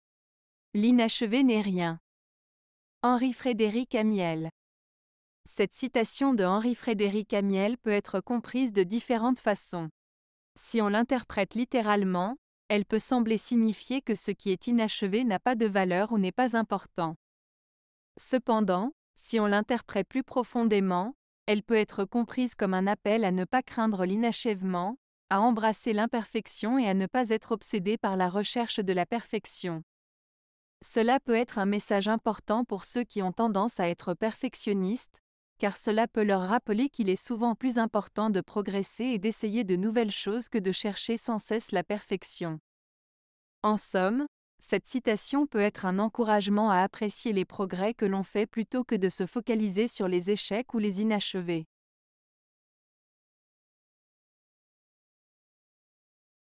L'inachevé n'est rien. - Henri-Frédéric Amiel
Cette citation de Henri-Frédéric Amiel peut être comprise de différentes façons. Si on l'interprète littéralement, elle peut sembler signifier que ce qui est inachevé n'a pas de valeur ou n'est pas important. Cependant, si on l'interprète plus profondément, elle peut être comprise comme un appel à ne pas craindre l'inachèvement, à embrasser l'imperfection et à ne pas être obsédé par la recherche de la perfection. Cela peut être un message important pour ceux qui ont tendance à être perfectionnistes, car cela peut leur rappeler qu'il est souvent plus important de progresser et d'essayer de nouvelles choses que de chercher sans cesse la perfection. En somme, cette citation peut être un encouragement à apprécier les progrès que l'on fait plutôt que de se focaliser sur les échecs ou les inachevés.